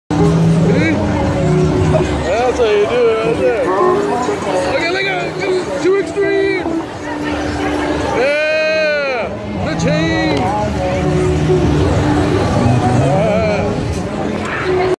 Work Together, Play Together
Our favorite ride @ the Oregon State Fair. We have made this an end of Summer tradition!